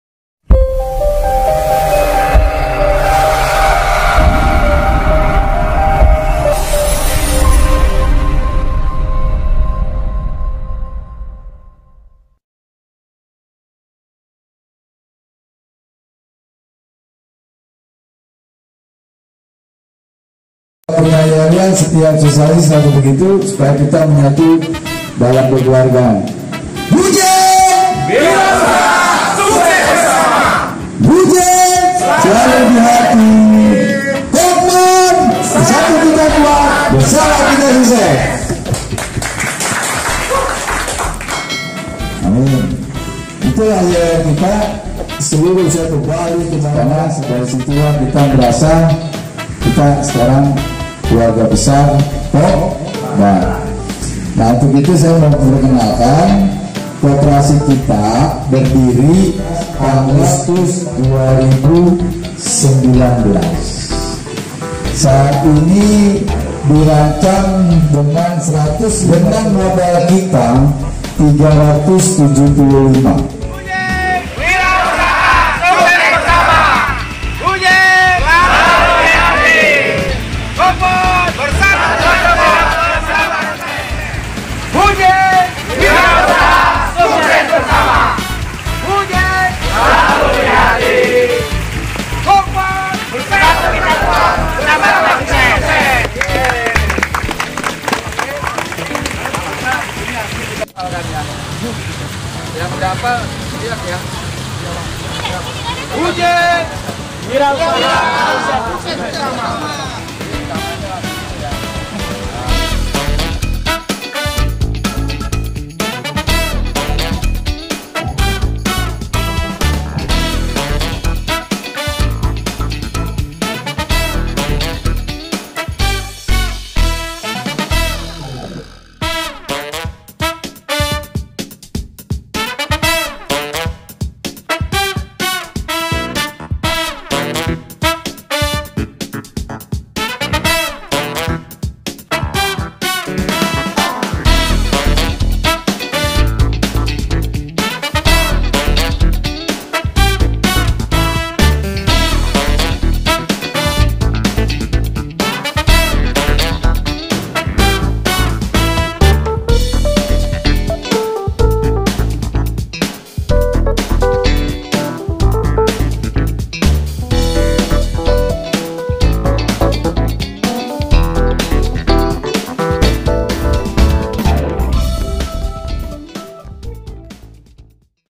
Beujek Kopban
Beujek Kopban adalah Ojol baru berbasis koperasi